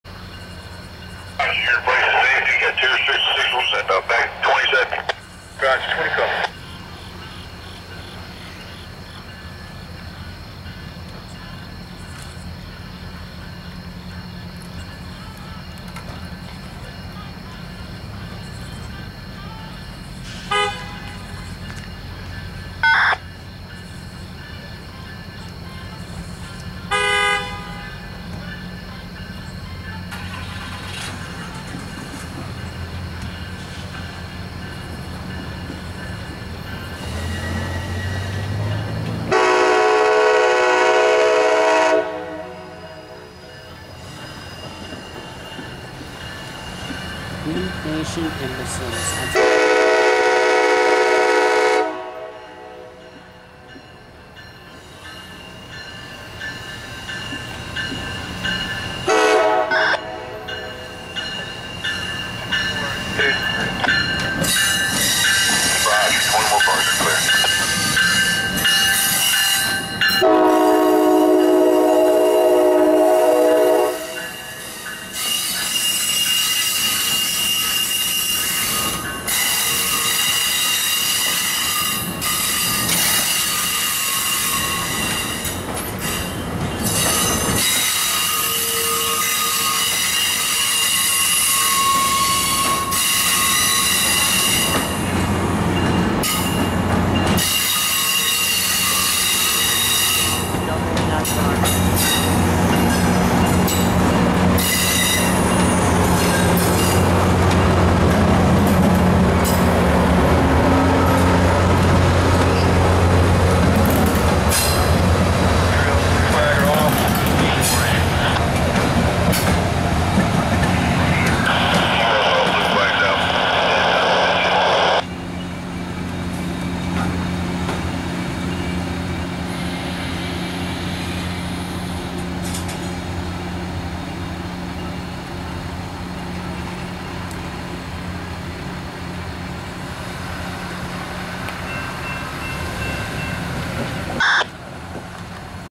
MARC P883 Reversing
MARC 883 finishing up the night reversing into the Yard